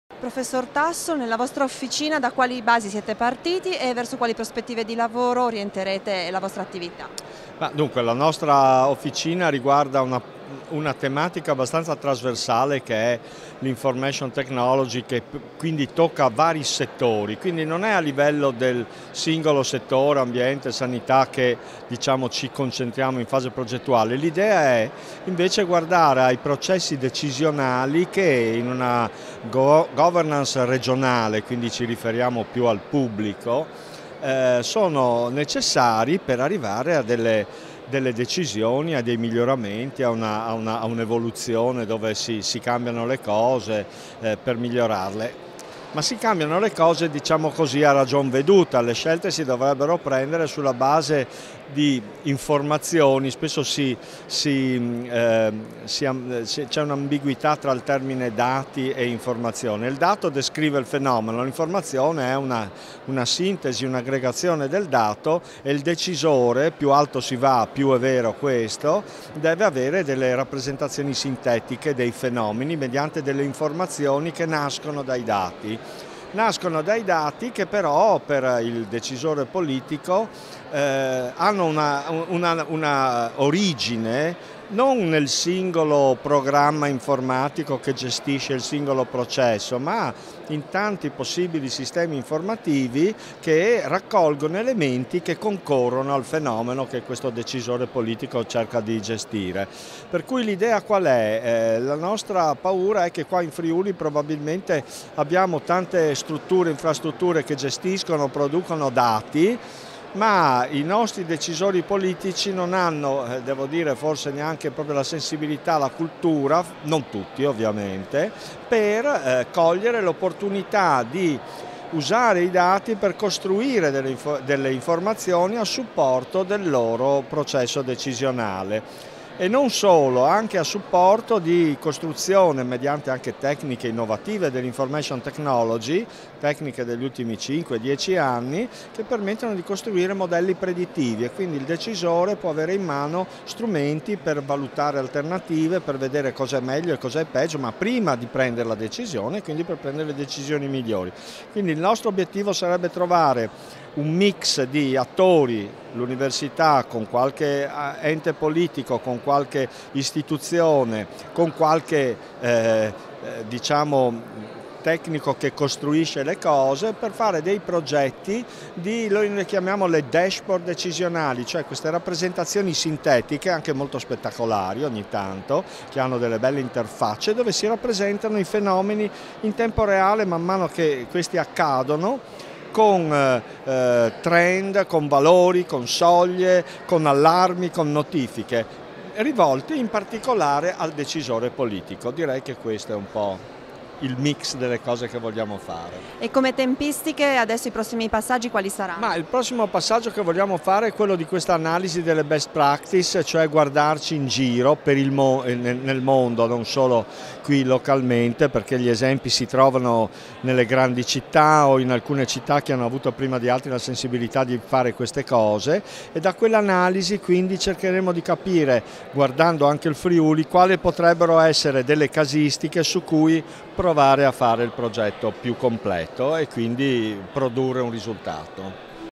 Cantiere Friuli: intervista a Carlo Tasso
Il referente dell’Officina “Sistemi digitali di supporto avanzato alle decisioni strategiche per il territorio” Carlo Tasso spiega obiettivi e finalità del lavoro che si svolgerà nei prossimi mesi.

Servizio a cura di Yes!Comunica